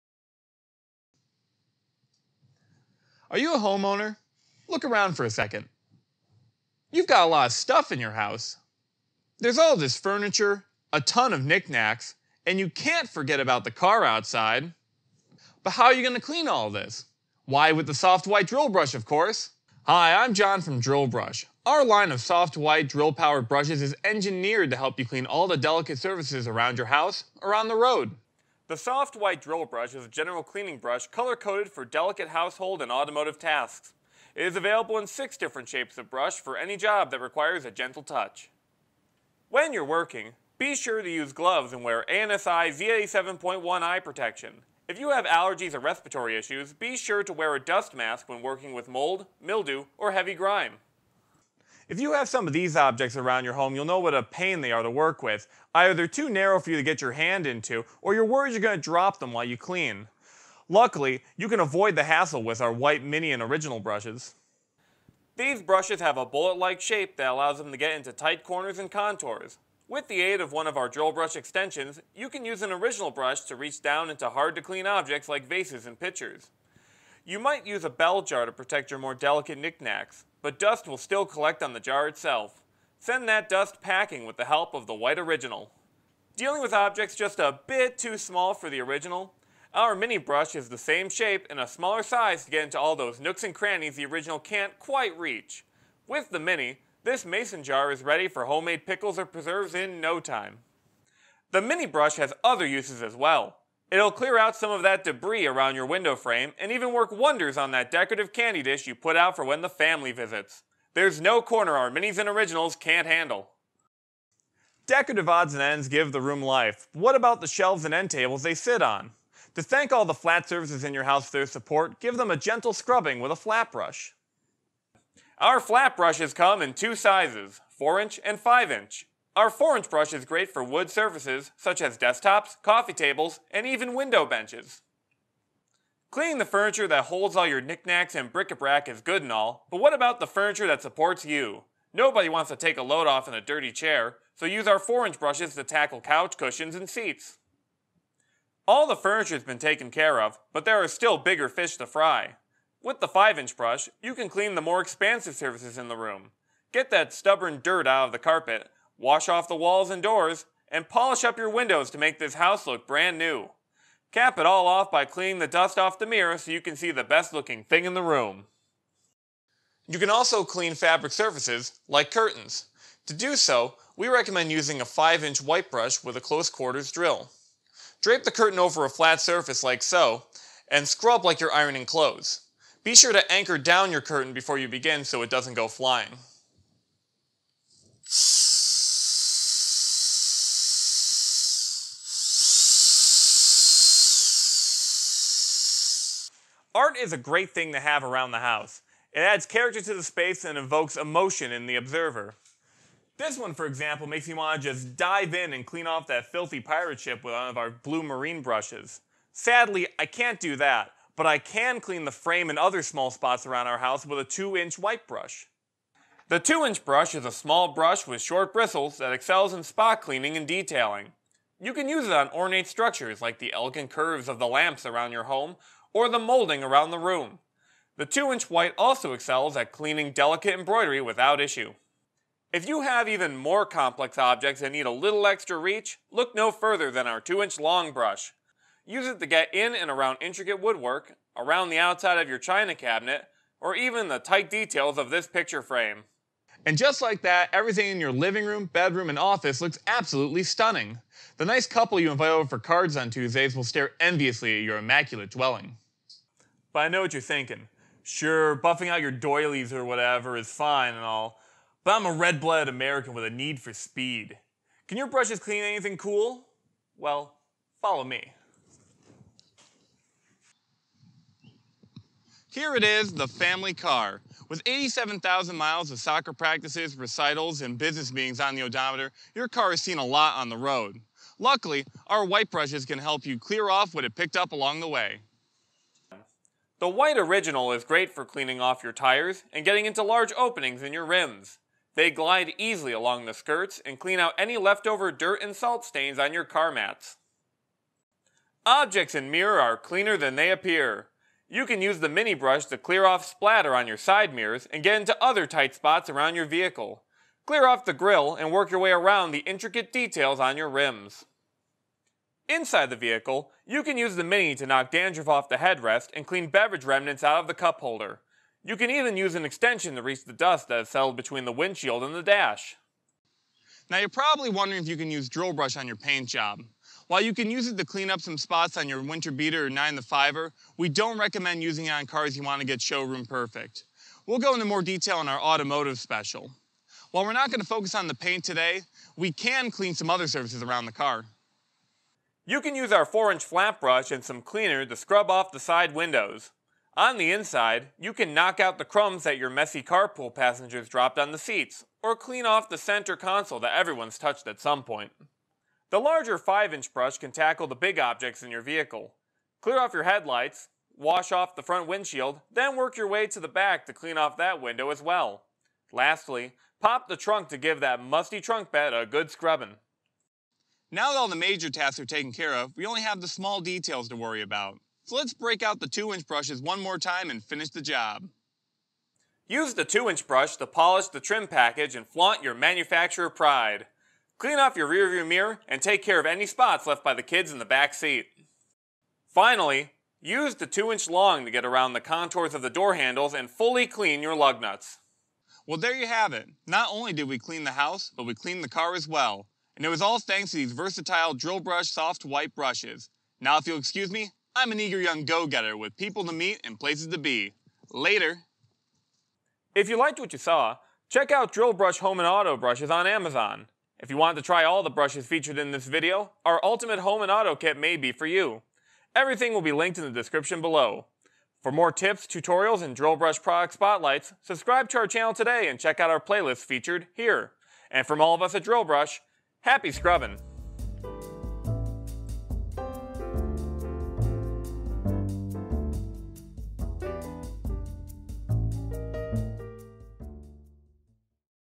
The ULTIMATE Home & Auto Cleaning Tools | Drillbrush Home & Auto Cleaning Guide
John from Drillbrush shows you how to clean your home and auto quickly and easily with Drillbrush White Soft drill-powered cleaning brushes!

Drillbrush Ultimate Home & Auto Kit

Credits:
Starring: John Cittadino, Jeff Evans
Writers - Russell Arcuri, John Cittadino, and Jeff Evans
Cinematography - Russell Arcuri
Color Grading - Russell Arcuri
Editor - John Cittadino
Thumbnail - John Cittadino

Timestamps:
0:00 Intro and Safety
0:51 Cleaning Vases, Pitchers, and More
1:54 Cleaning Household Surfaces
3:16 Household Detail Cleaning
4:06 House Cleaning Wrap-up
4:31 Automotive Intro
4:46 Cleaning In and Around the Vehicle
5:23 Cleaning Auto Surfaces
6:10 Auto Detailing
6:36 Outro